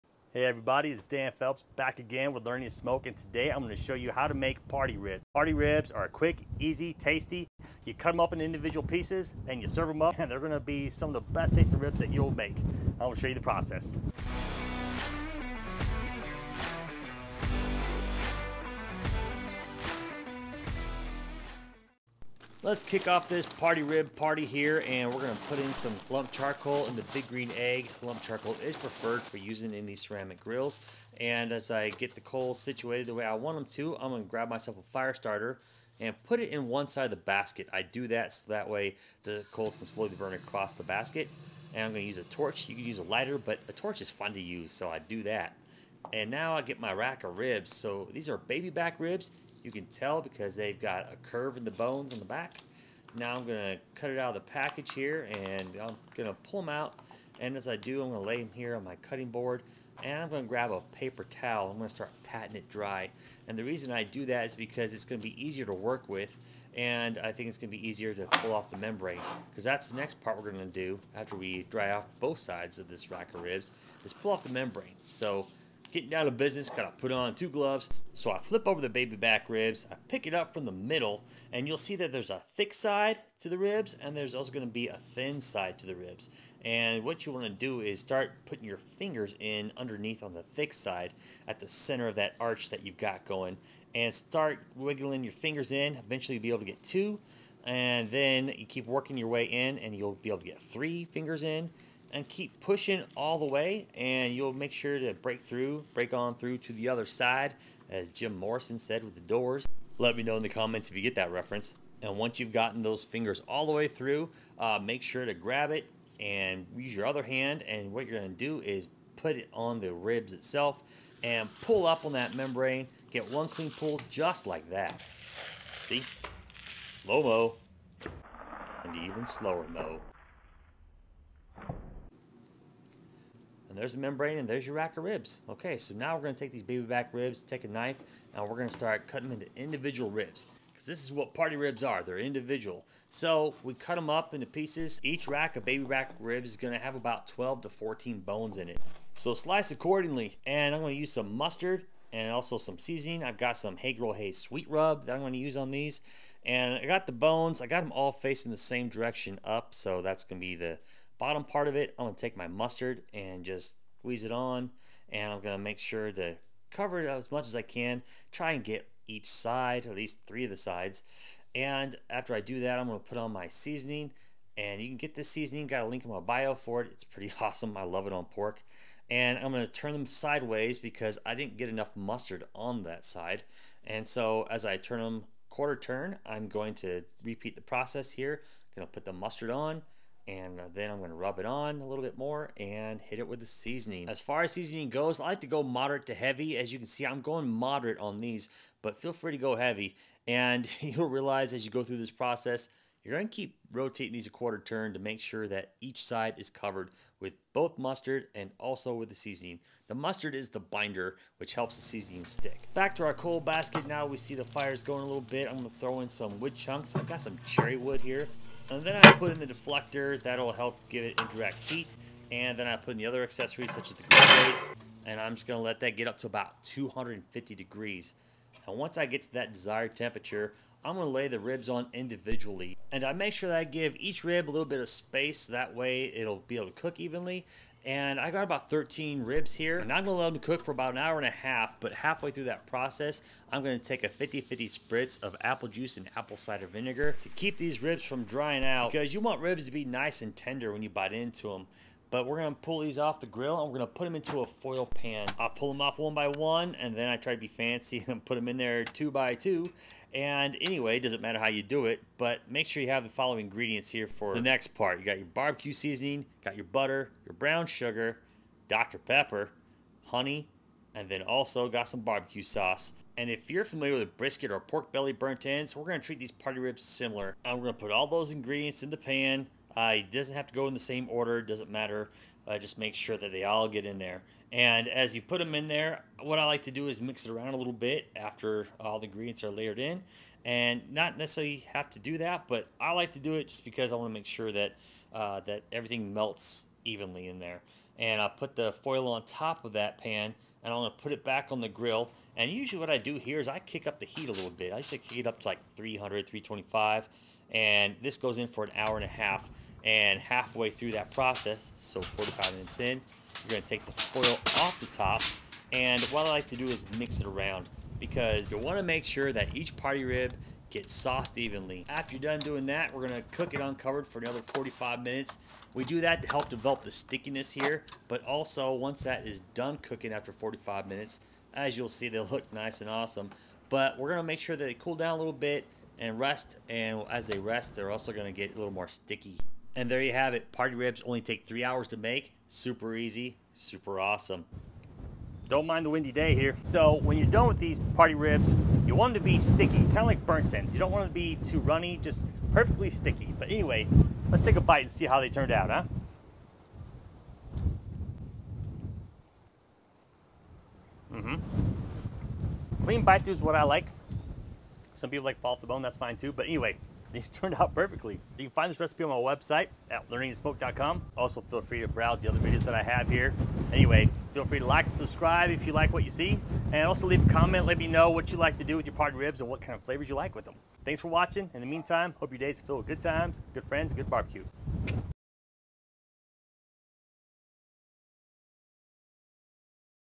The Ultimate Smoked Party Ribs Recipe!
Smoked party ribs are the viral sensation in which ribs are cut into individual pieces and smoked, giving you that soft crust and saucy flavor on all four sides (instead of two). They cook faster this way, in only three hours. Serve them up for a party or for yourself!

In this party ribs recipe video, you will learn the following:

• Removing the membrane from baby back ribs
• Cutting ribs
• Ingredients
• Seasoning
• Smoking
• Spritzing
• Wrapping in foil pan
• Serving

Side note: it got very windy while filming, thus making the camera wobbly.

MY FAVORITE BBQ TOOLS

00:00 Intro
00:45 Removing Membrane from Baby Back Ribs
02:16 Cutting Party Ribs
02:30 Binder and Seasoning
03:32 Smoked Party Ribs
04:20 Ribs in Foil Pan
05:43 Taste Test
06:08 Outro